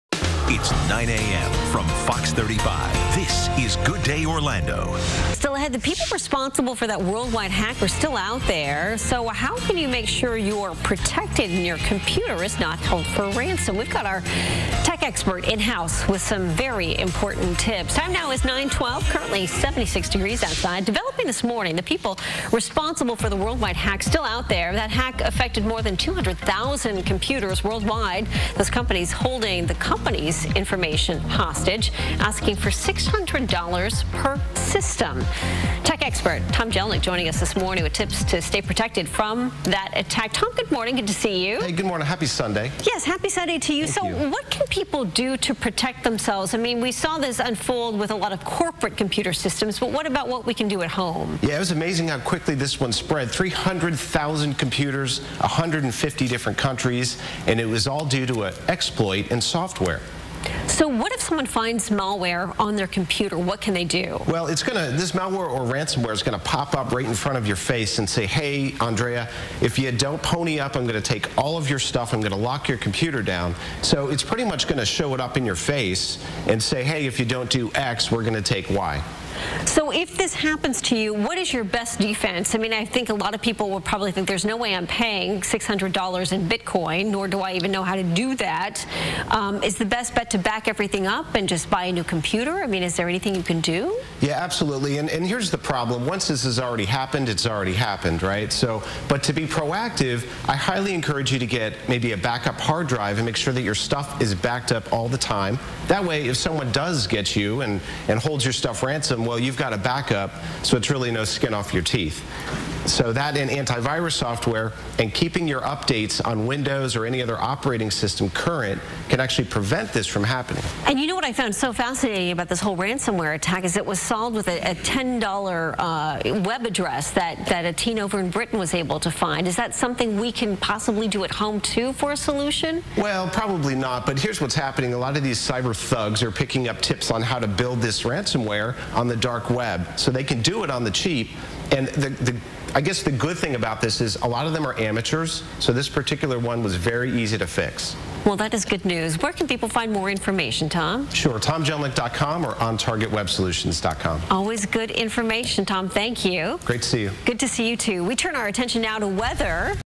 Some Tips To Help Prevent Ransomware From Tom Jelneck
Orlando tech & digital marketing expert Tom Jelneck discusses ways to prevent and protect yourself from Ransomware. Jelneck is founder & CEO of On Target Web Solutions, an Orlando digital marketing agency.